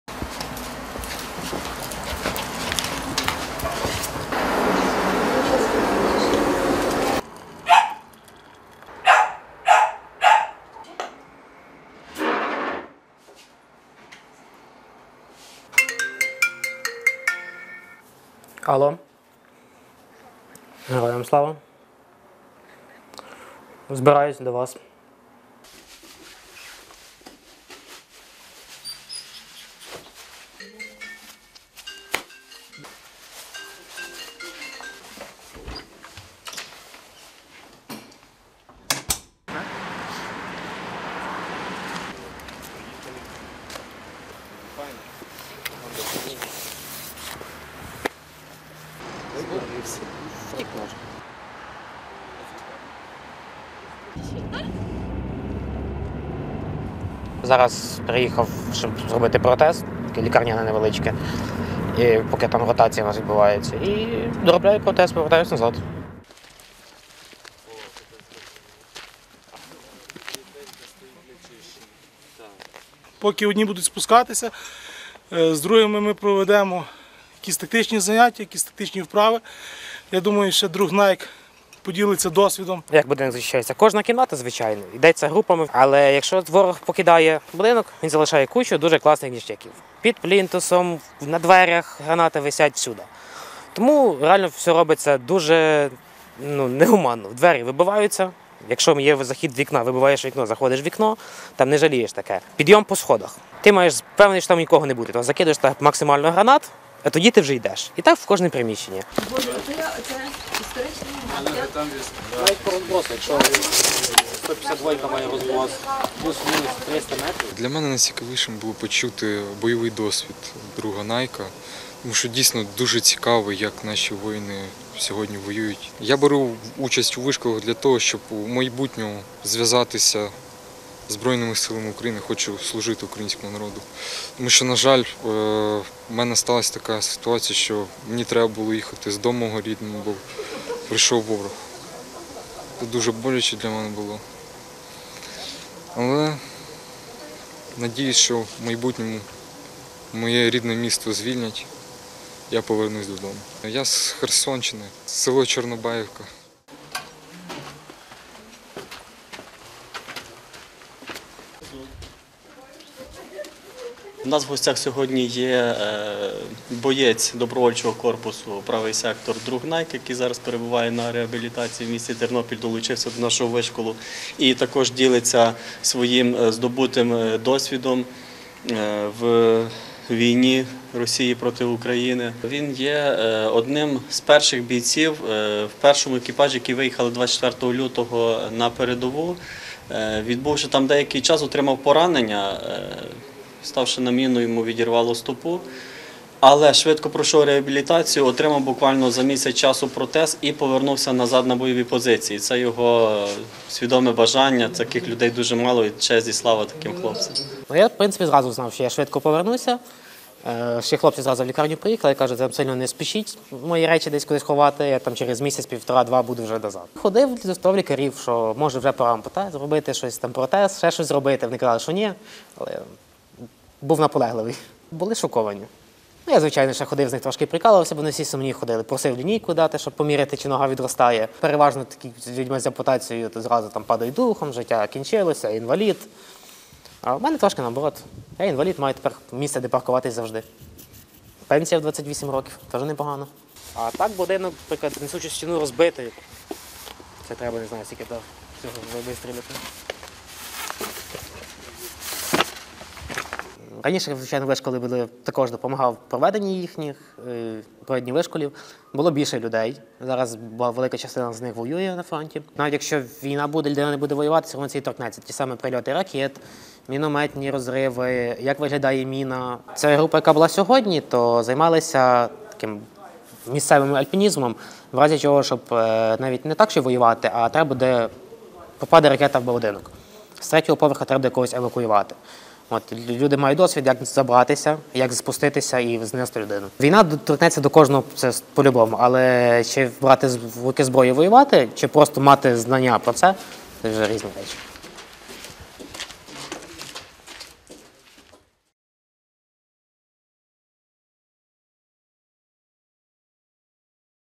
Військовий Іван Тима вишколює молодь в Тернополі, поки чекає на новий протез
Військовий Іван Тима у червні підірвався на протипіхотній міні та втратив ногу. Він відмовився від реабілітації і за кілька тижнів повернувся до виконання бойових завдань. Зараз приїхав зі Сходу у Тернопіль, щоб замінити протез. Про поранення, війну та вишколи для молоді він розповів Суспільному.